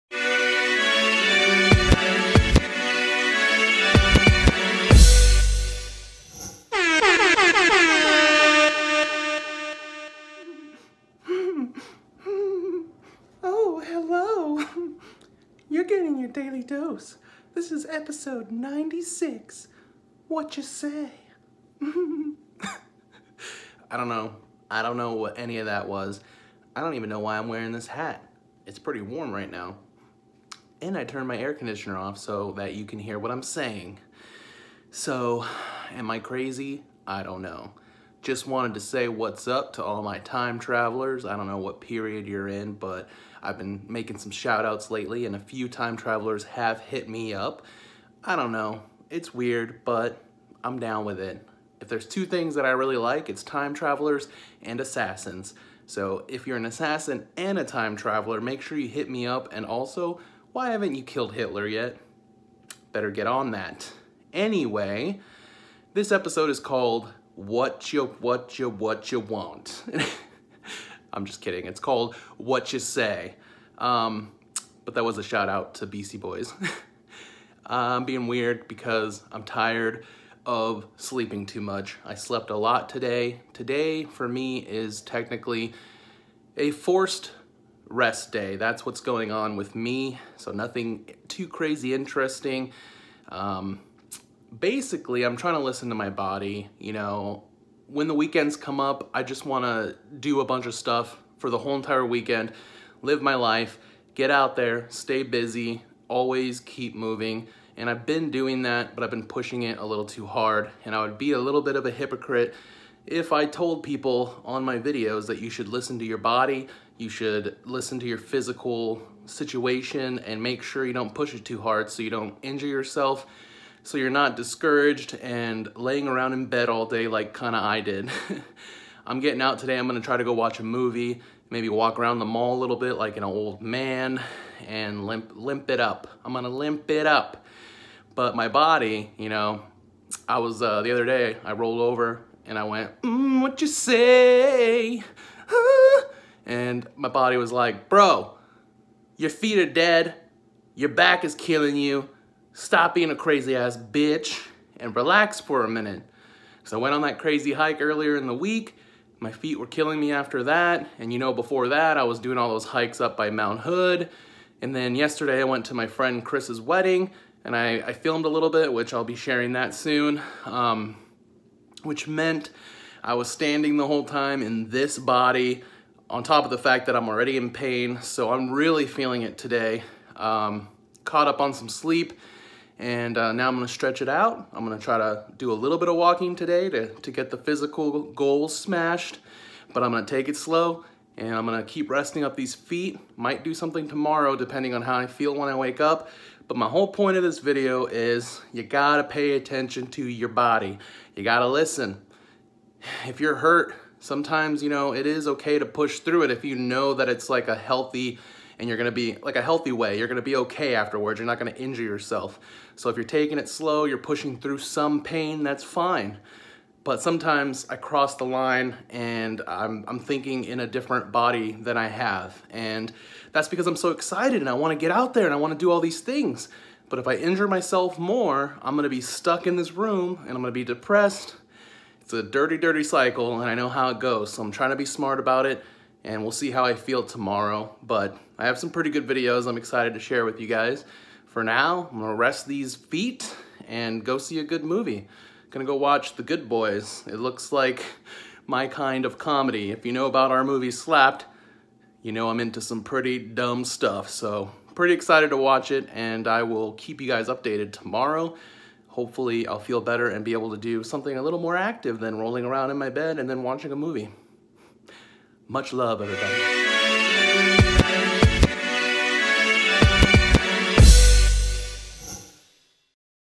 Daily Dose of LoweTivation - Episode 96
Normally I would feel way, way down about how lazy I’ve been today but I’ve reminded myself that I’m still over 450 pounds and I’ve been pushing it hard like I’m in a skinny man’s body haha.

So I have these injuries in my feet and back that I can’t simply keep ignoring or they will become worse and I won’t be able to move at all. So this weekend is about being a bit smarter and resting up. Still gonna get my walking goals in but not anything more than that and doing some therapy for my body! Much love everyone and always remember to listen to your body!❤️injury